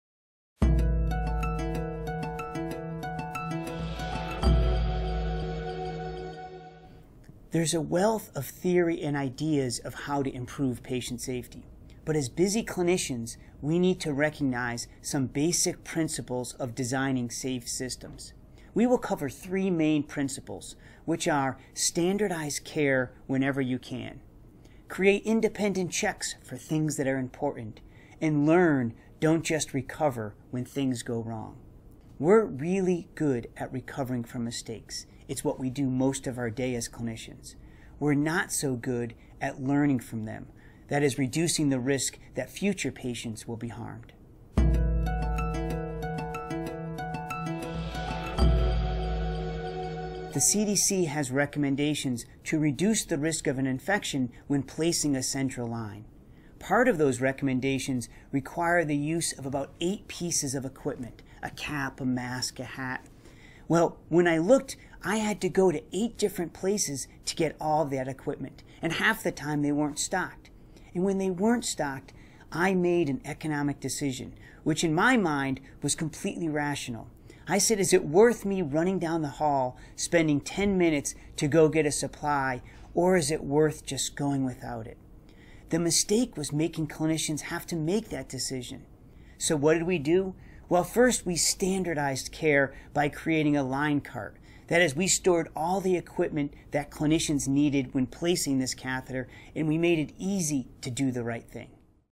CUSP: Standardize When You Can
AHRQ, CUSP, Making it easy to do the right thing helps busy clinicians follow proven principles of safety.  See how this CUSP technique is applied in a busy ICU to safely remove catheters.